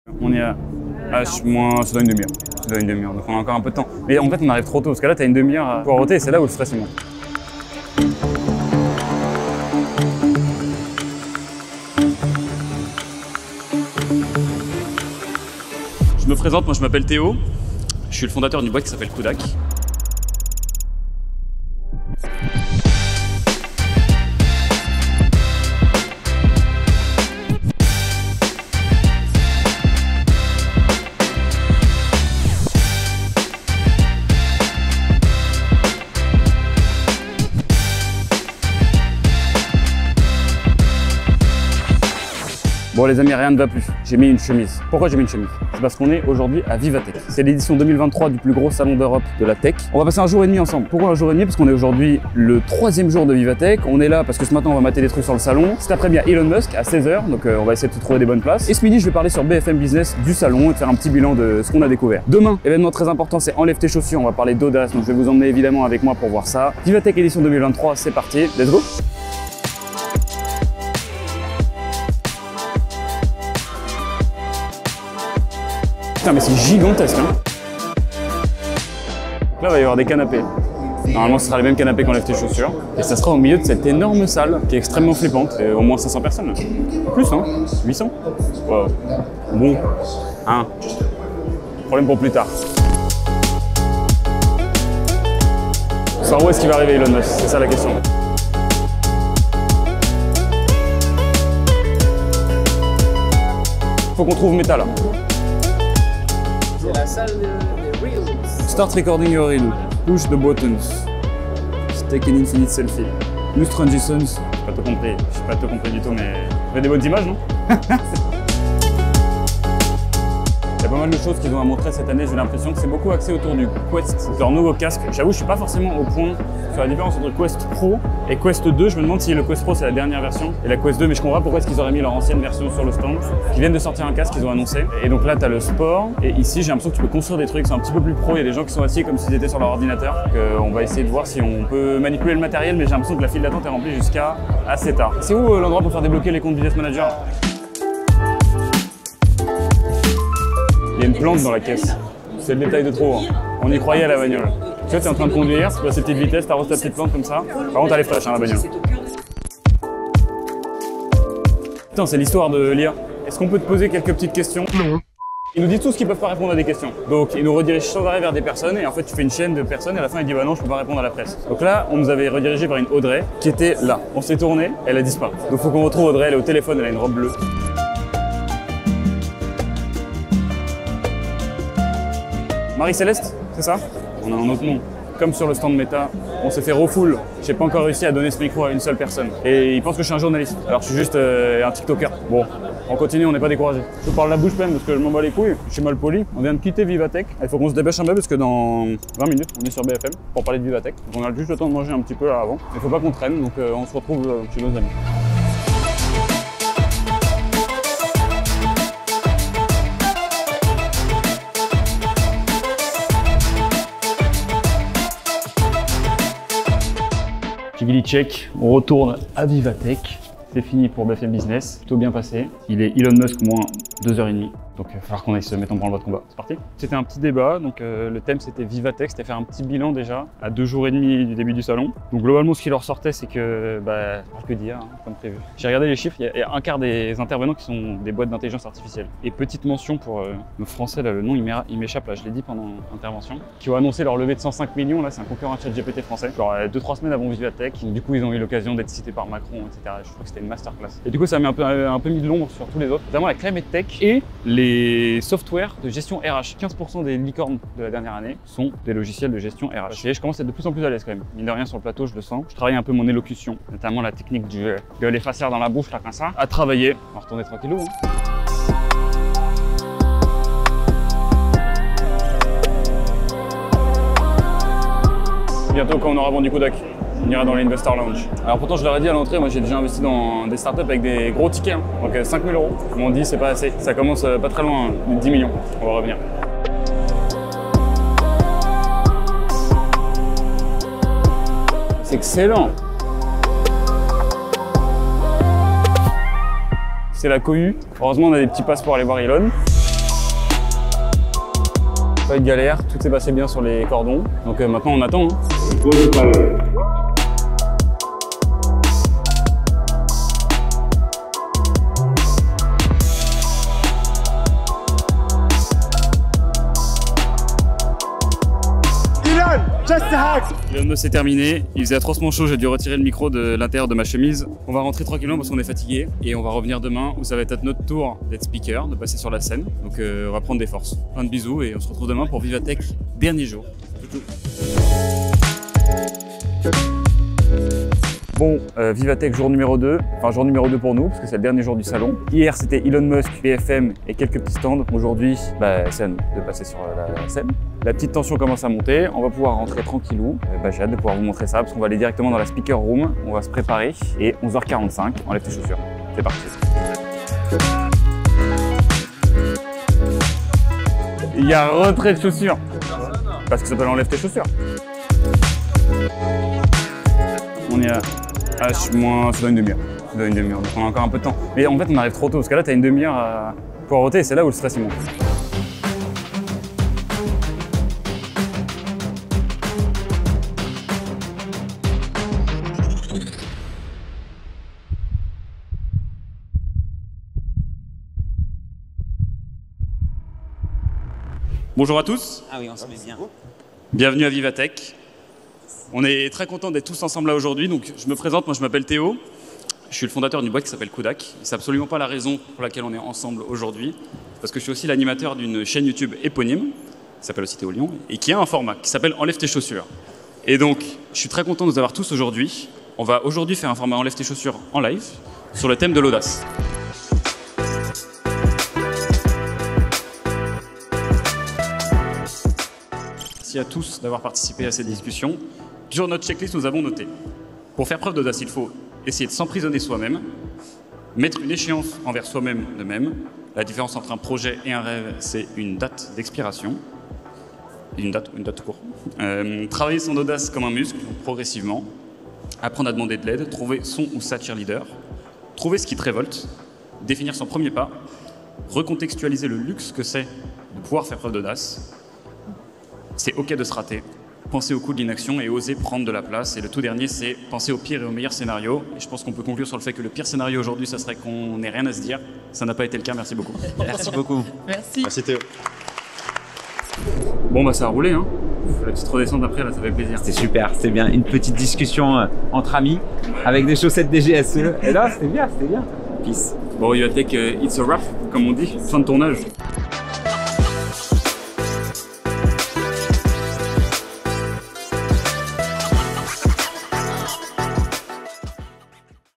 Ma conférence a Vivatech (Enleve tes chaussures)
Je vous emmène pour deux jours survoltés à VivaTech, le temple de l'innovation et des start-up ! 💻

Au programme : Visite des stands, passage sur BFM, l'intervention épique d'Elon Musk. Mais surtout, j'ai réussi à ramener Enlève tes chaussures à VivaTech en direct et face à un public enflammé et ça, c'est fou !

- CHAPITRAGE -
00:00 : Intro
01:09 : Visite des stands – Part.1
03:26 : Passage sur BFM
04:28 : Visite des stands – Part.2
04:56 : Elon Musk
05:35 : Enlève tes chaussures
07:56 : Conclusion

//🟡Pour bosser avec Coudac ça se passe ici :
5 expertises, 5 agences made in Coudac.